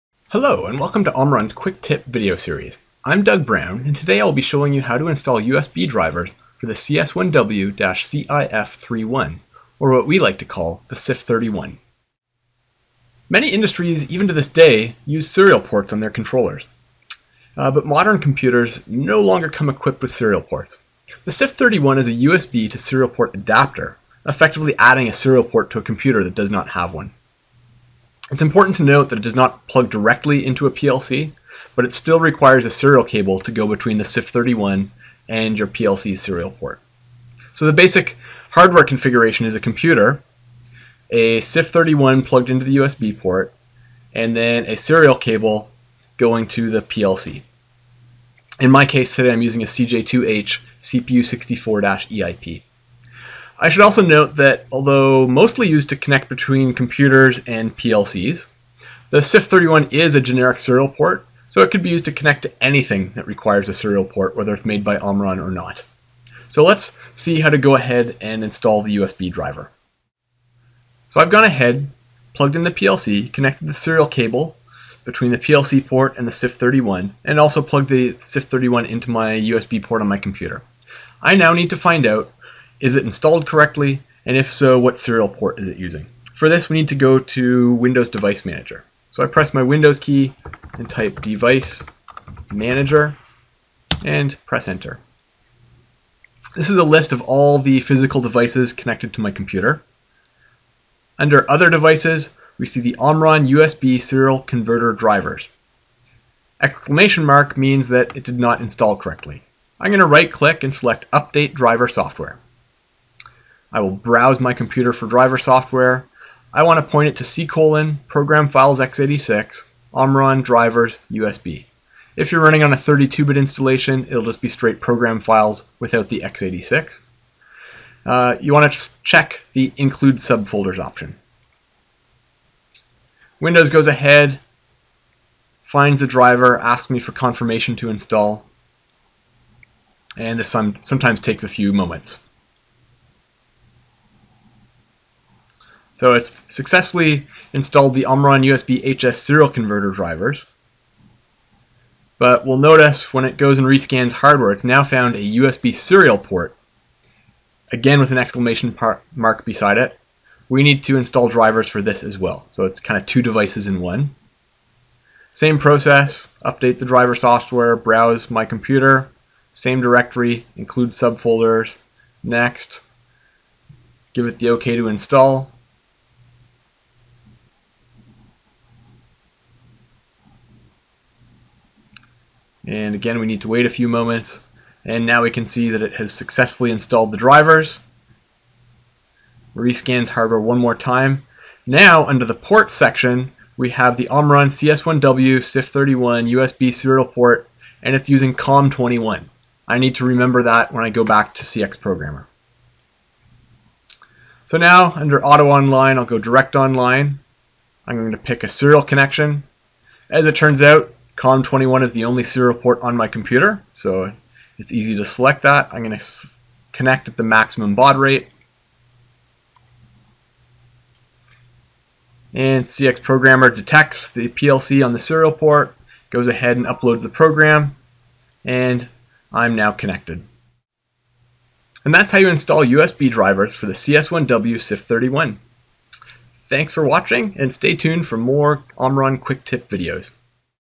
CS1W-CIF31 USB Driver Installation
Quick Tip Video - How to install the CS1W-CIF31 USB Driver